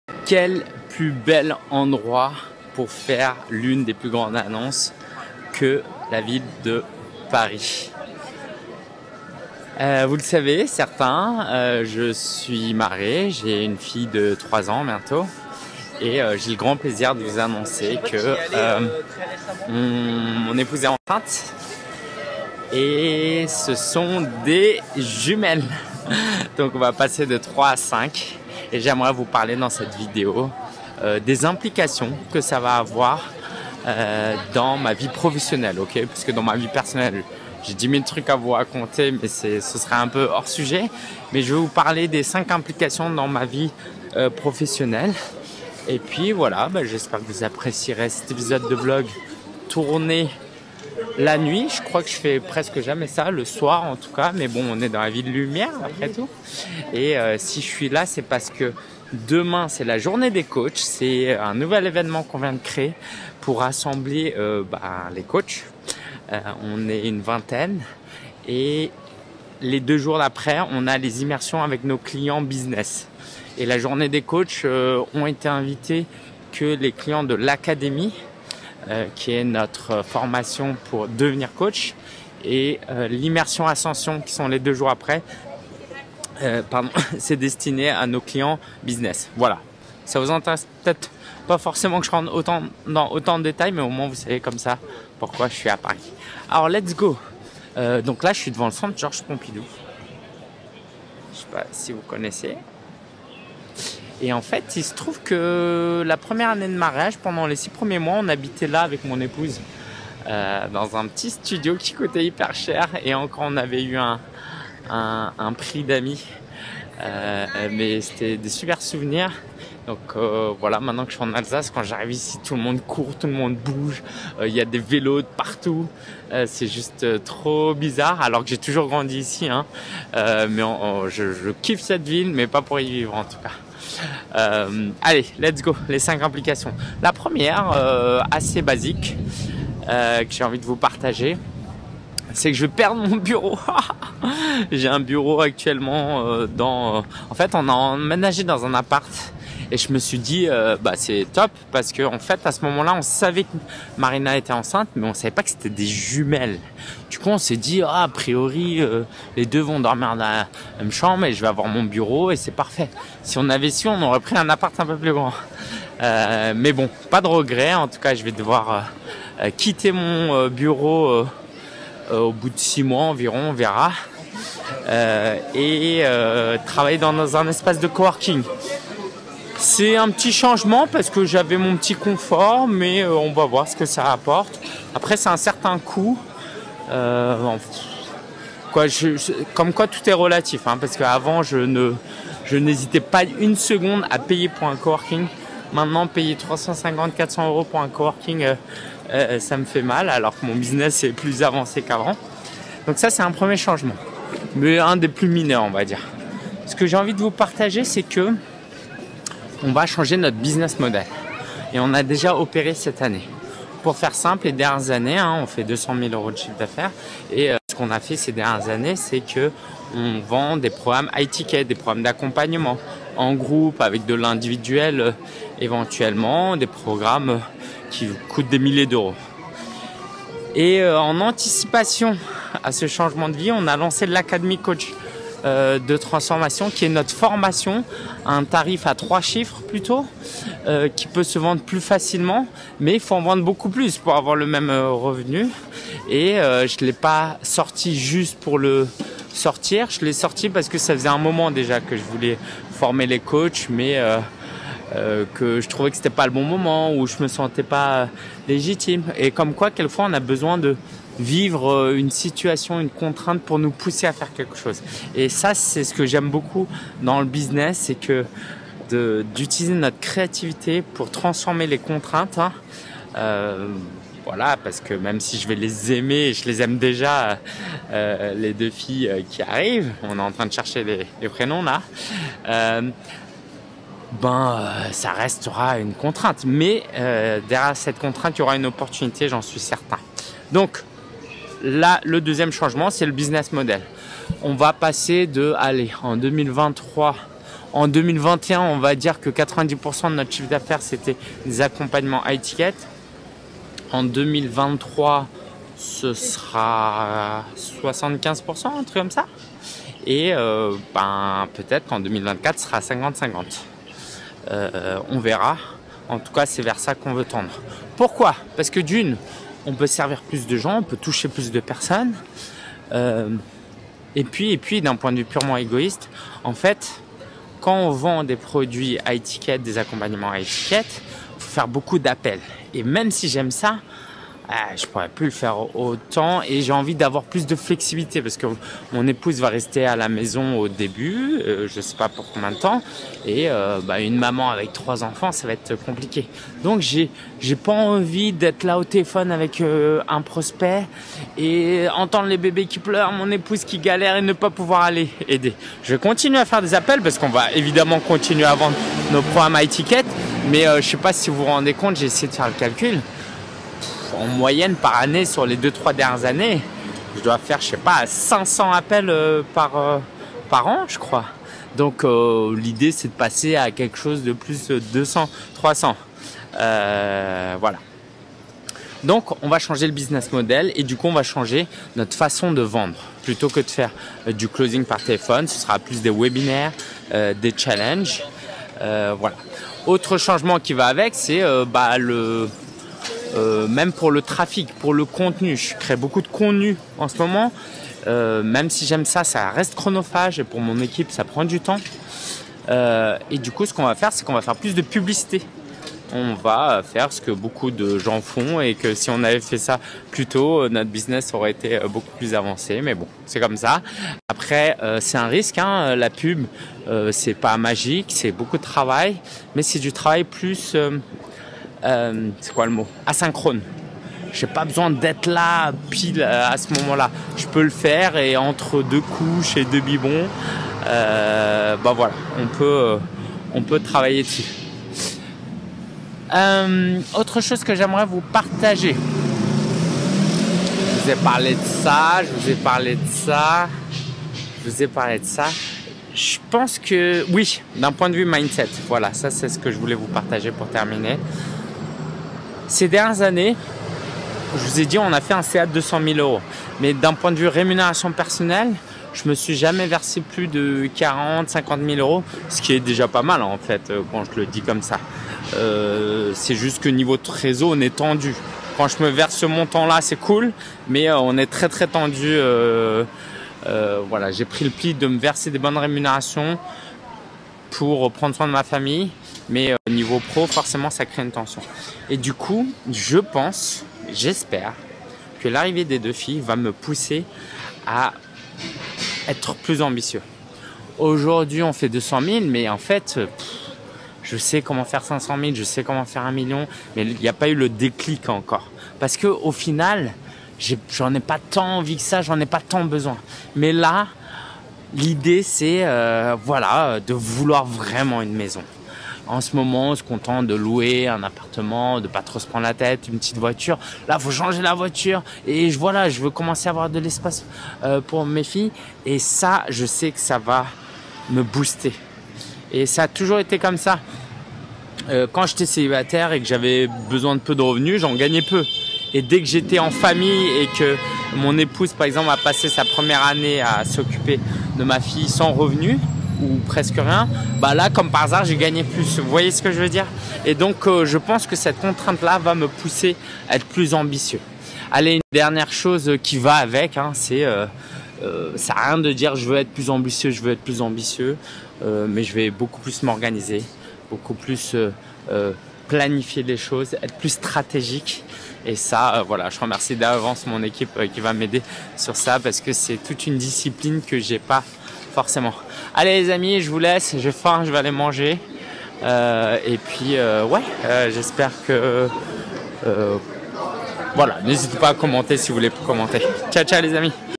FAMILLE QUI S'AGRANDIT = 5 CHANGEMENTS IMMÉDIATS | Vlog n°246
🎓 Envie d’investir en toi et atteindre tes objectifs ?! Découvre nos programmes dédiés aux Coachs

Des jumelles 👯‍♀️ ? Ce qui va changer !

En 2024, ma vie va être chamboulée.

Notamment dans ma vie pro comme je l’anticipe dans ce Vlog.

Si tu es parent, dis-moi en quoi ta vie pro serait ralentie par tes enfants.

Je suis curieux :)

Ling-en